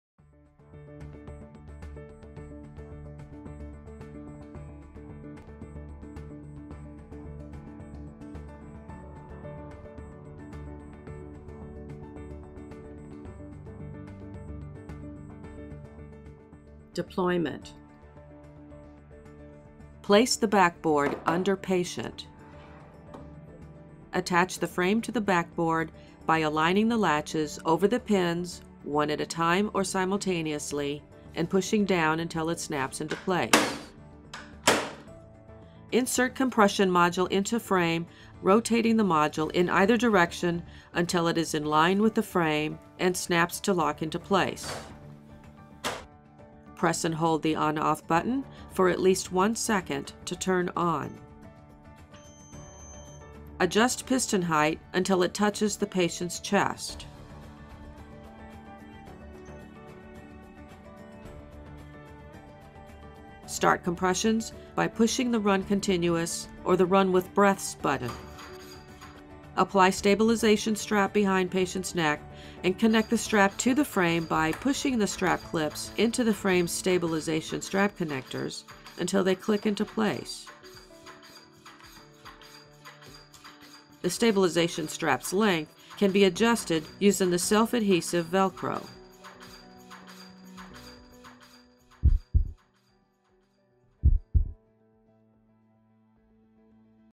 US Defibtech Lifeline ARM Deployment (Step 1)
The Defibtech Lifeline ARM is an automated, portable, battery-powered device that provides chest compressions on adult patients who have cardiac arrest, and is intended for use by qualified medical personnel certified to administer CPR. Deployment is the first step in using the Lifeline ARM.